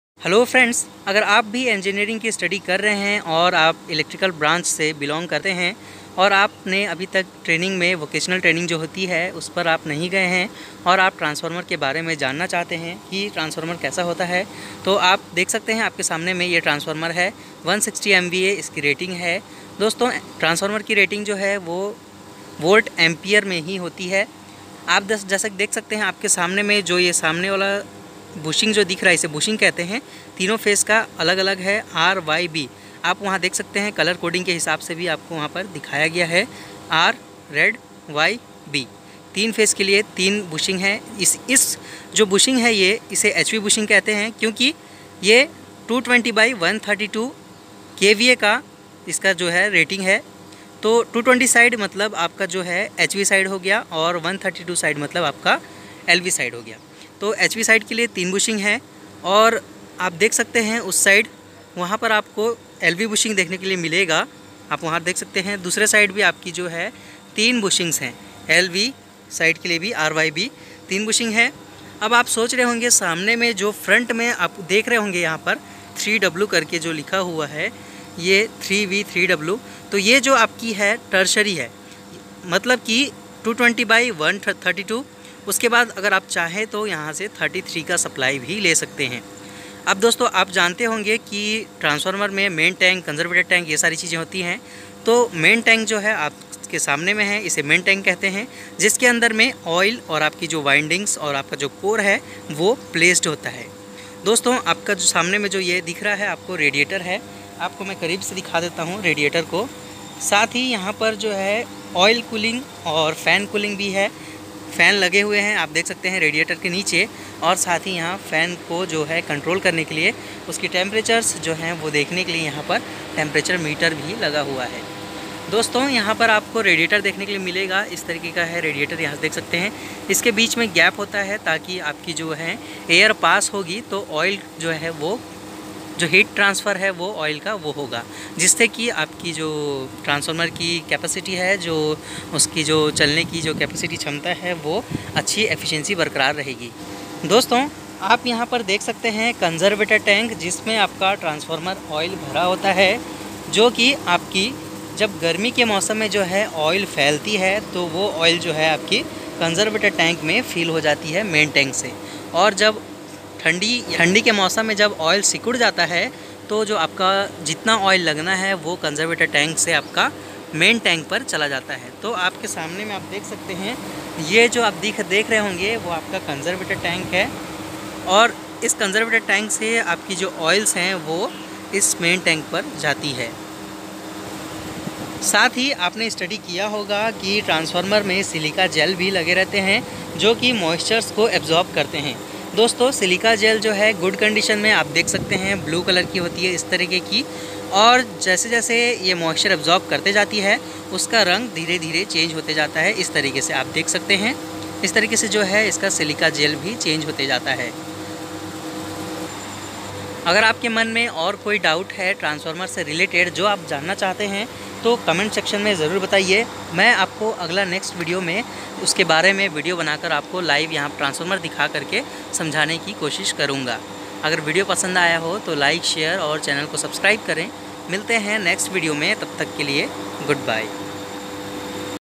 Watch 160 MVA Transformer practically | Bushing | Conservator tank | main tank | Radiator
hope informative for you